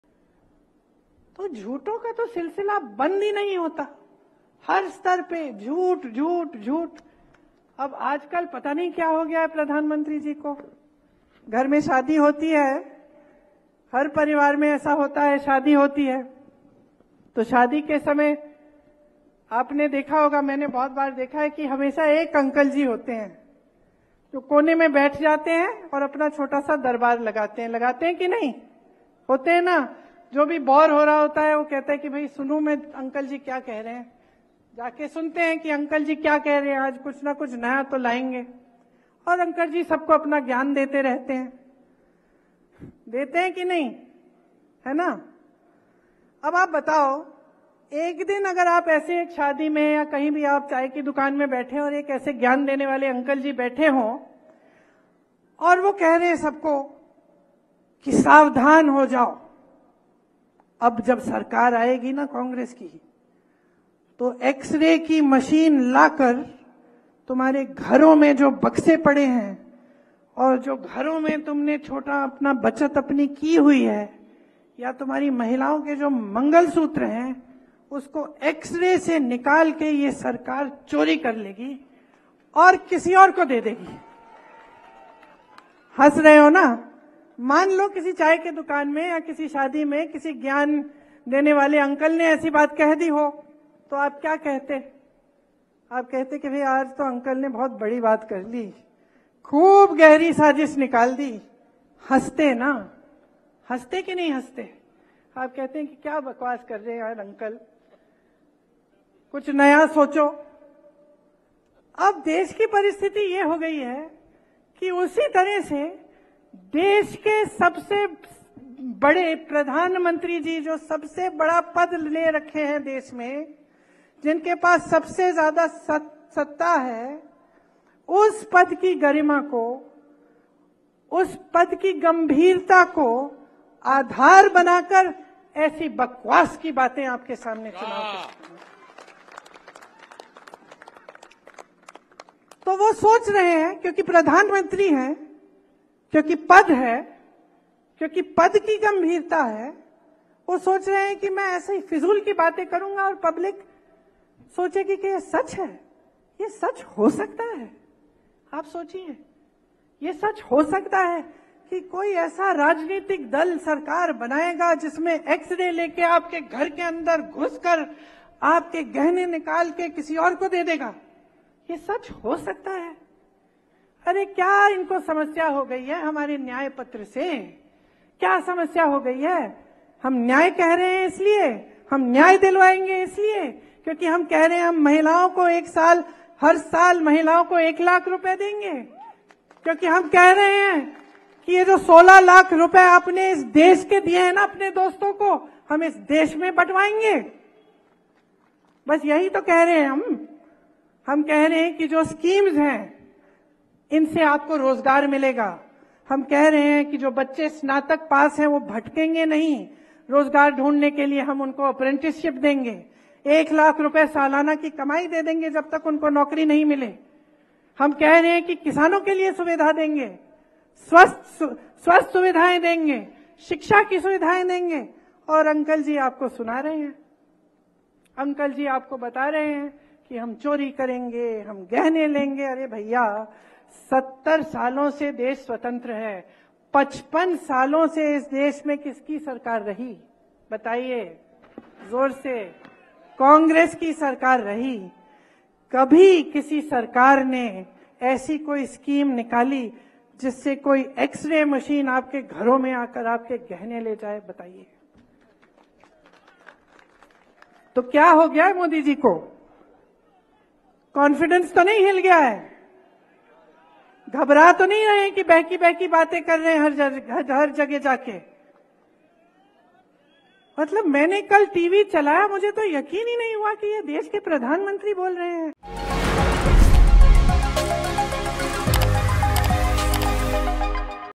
Mangalsutra Controversy: प्रियंका ने पीएम मोदी को कहा ‘अंकल’,मंगलसूत्र विवाद पर बीजेपी को घेरा
कांग्रेस महासचिव प्रियंका गांधी ने गुजरात के वलसाड में एक जनसभा को संबोधित करते हुए अपनी पिता राजीव गांधी को याद किया। उन्होंने पूर्व प्रधानमंत्री दिवंगत राजीव गांधी को याद किया और कहा कि मैं राजीव गांधी को टुकड़ों में घर लाई। मुझे पता है कि देश के प्रधानमंत्री कैसे काम करते हैं।